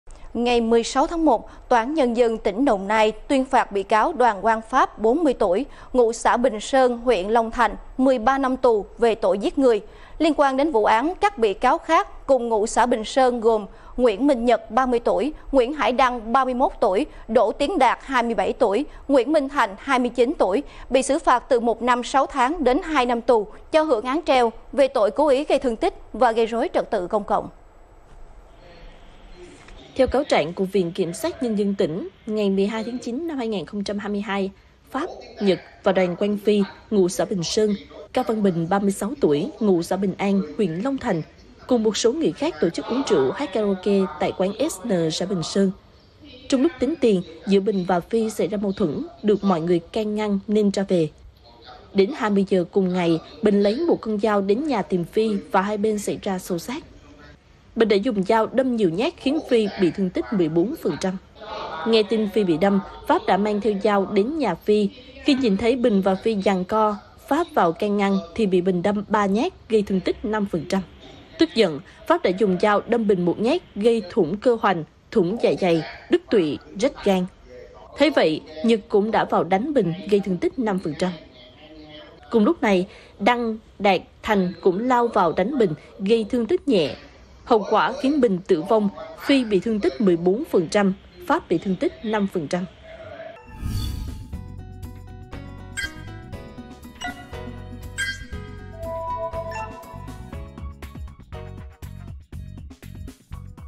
Người đàn ông lãnh án giết người do mâu thuẫn trong lúc tính tiền nhậu | Tin tức 24h mới nhất | ANTV
ANTV | Tin tức 24h mới nhất | Ngày 16/1, Tòa án Nhân dân tỉnh Đồng Nai mở phiên tòa xét xử bị cáo Đoàn Quang Pháp và các đồng phạm liên quan trong một vụ án giết người do mâu thuẫn.

Mời quý vị và các bạn xem thêm: Tội ác động trời những con quỷ khát máu cuồng sát người rồi đào hố phi tang sau vườn